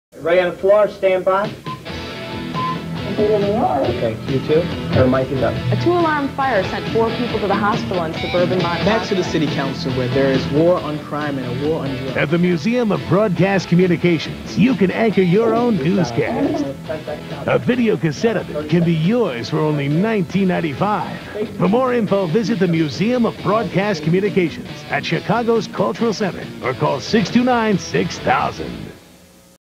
Museum Of Broadcast Communications | Television Commercial | 1992
The Museum of Broadcast Communications (MBC) is an American museum, the stated mission of which is "to collect, preserve, and present historic and contemporary radio and television content as well as educate, inform and entertain through our archives, public programs, screenings, exhibits, publications and online access to our resources."[1] It is located in Chicago, Illinois.